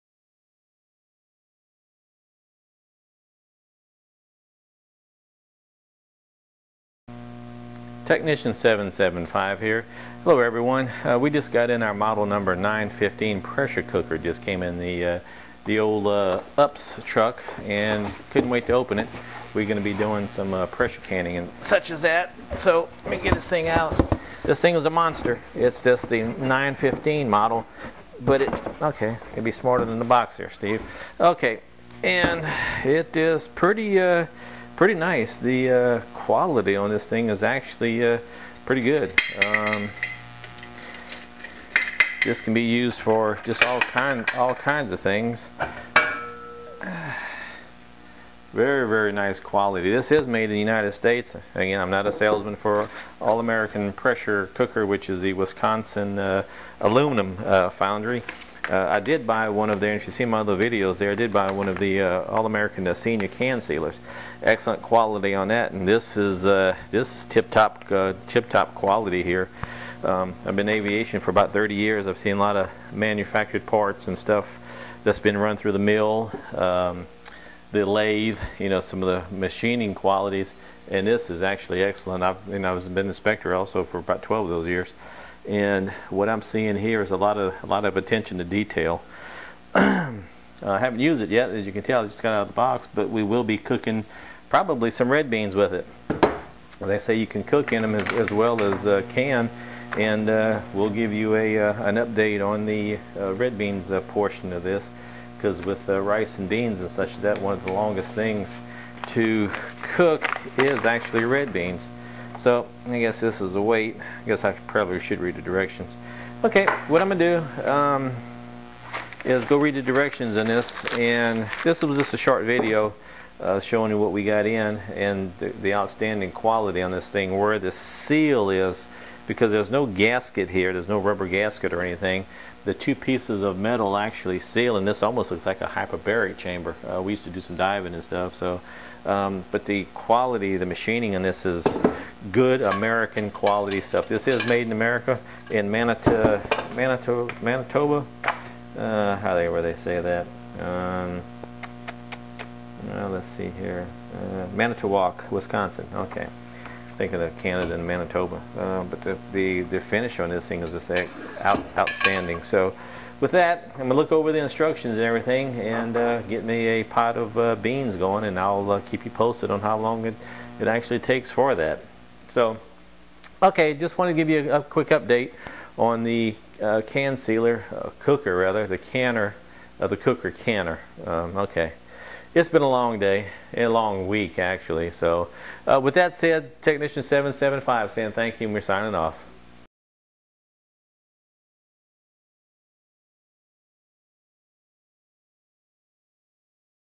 Equipment: Pressure Cooker/Canner Inspection
In this video, we inspect a new All American Pressure Cooker model # 915. The quality is superb. I did find out we can not cook beans or rice in here. :( but bacon we can :)